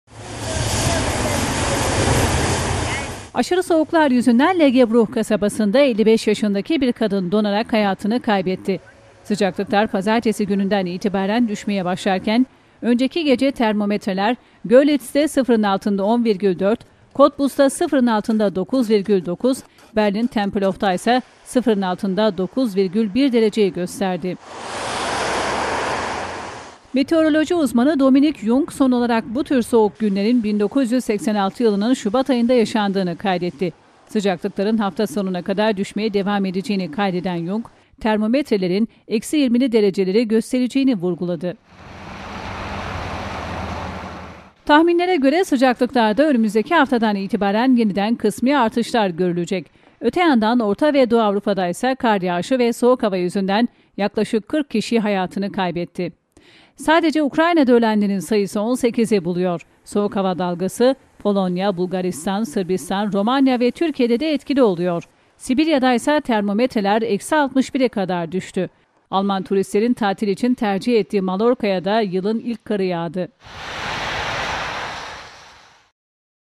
Aşırı soğuklar can kaybına yol açtı
ALMANYA SON 26 YILIN EN SOĞUK GÜNLERİNİ YAŞIYOR